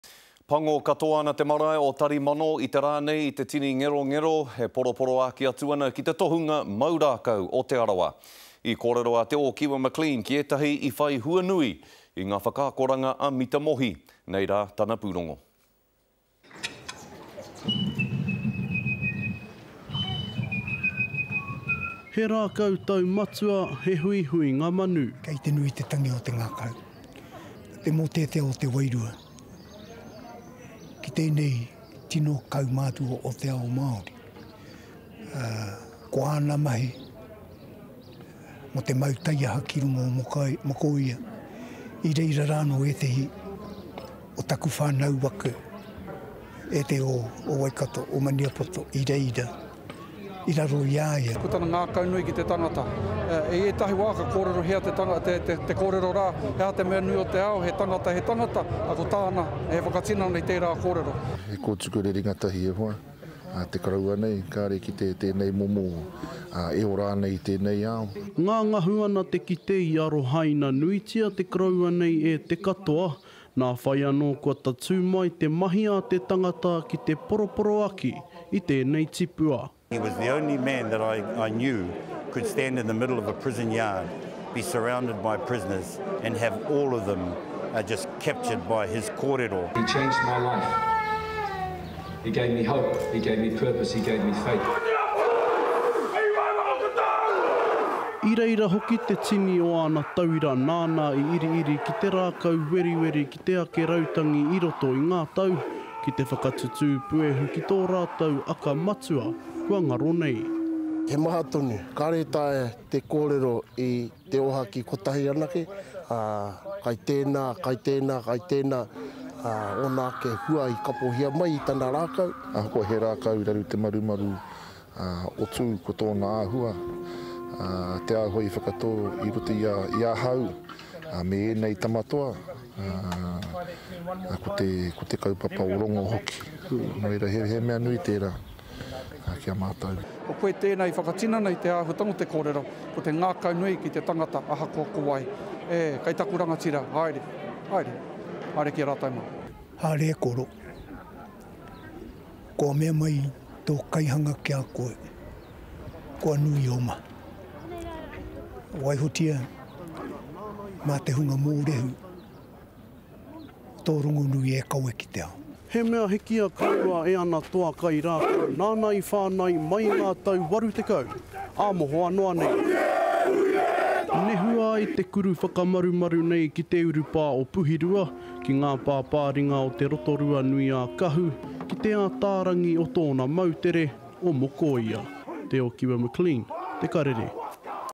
Masses converge on marae paying respect to Mita Mohi
The masses gathered at Tarimano Marae today to farewell Te Arawa's mau rākau exponent. Te Okiwa McLean was there and caught up with a few people who were inspired by this role model. Here’s his story.